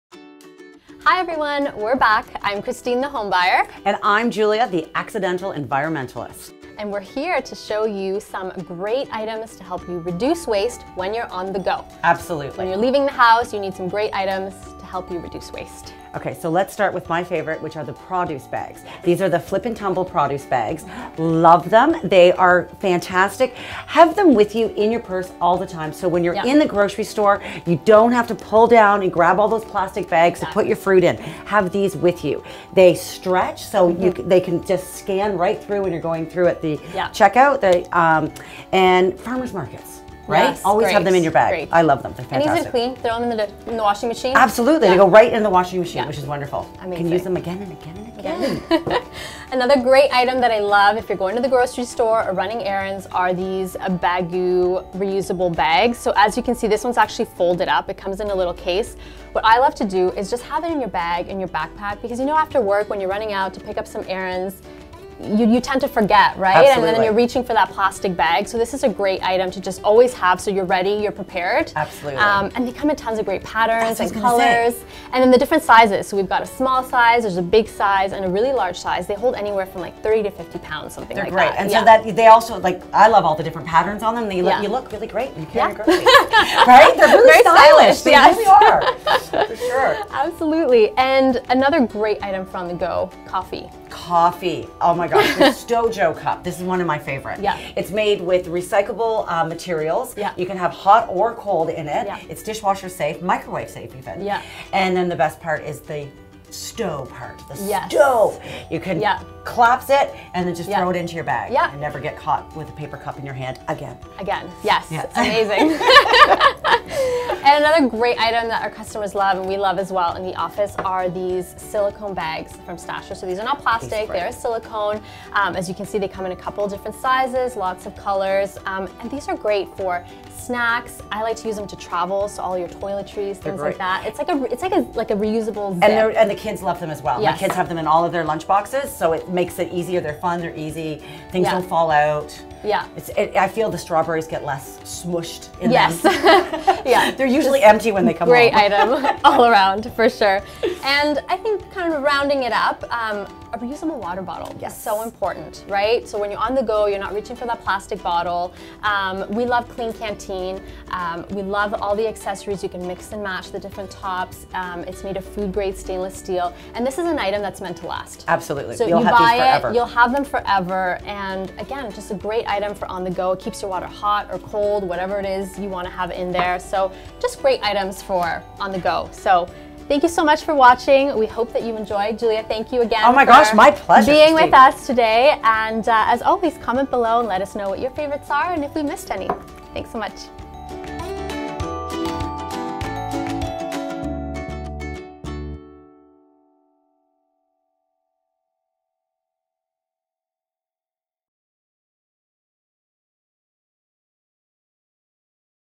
Everything You Need to Reduce Waste - Part 2
On a journey to make better choices and reduce waste in your daily life? We’ve got you covered with some of our zero waste essentials to get you started.

3. Stojo Collapsible Biggie Cup
5. Klean Kanteen Classic Bottle Sport Cap